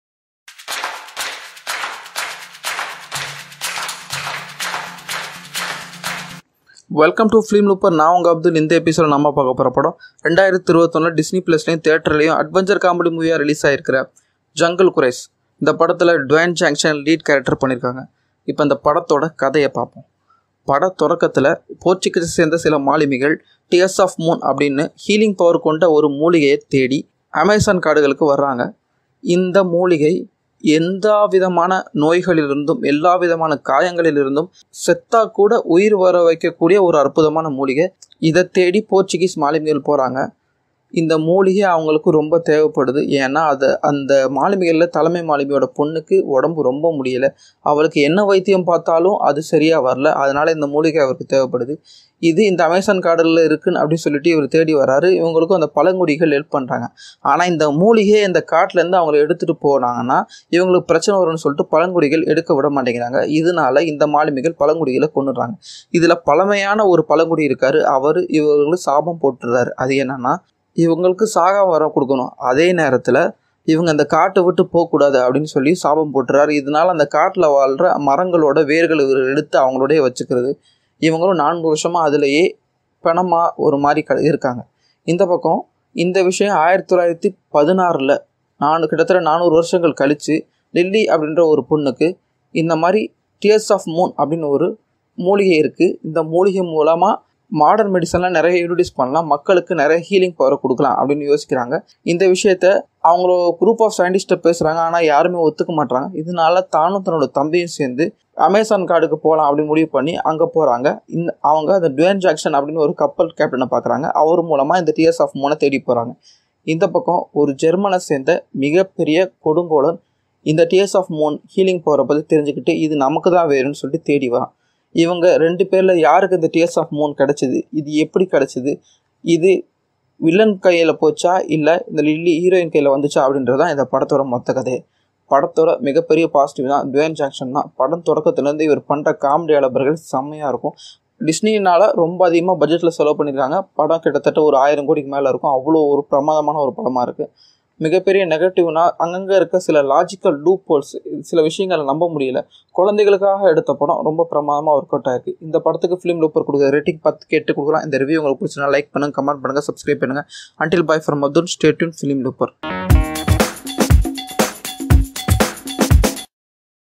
Jungle Cruise (2021) review in tamil by abdul|dwayne jhonson|Disney|filmlooper
ased on Disneyland's theme park ride where a small riverboat takes a group of travelers through a jungle filled with dangerous animals and reptiles but with a supernatural element cast:Dwayne Johnson & Emily Blunt junglecrusie disney dwanybe johnson